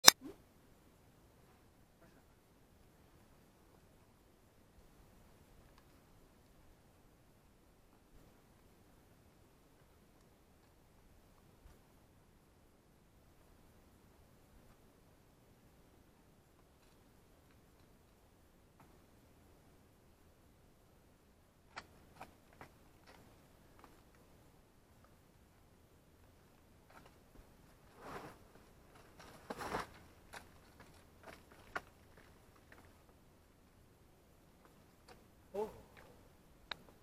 Fisicalsaurus, 6a. Can Camps
Climber: Xema Sanz
Name: Fisicalsaurus
Grade: 6a
Zone: Can Camps
Boulder: Fisicalsaurus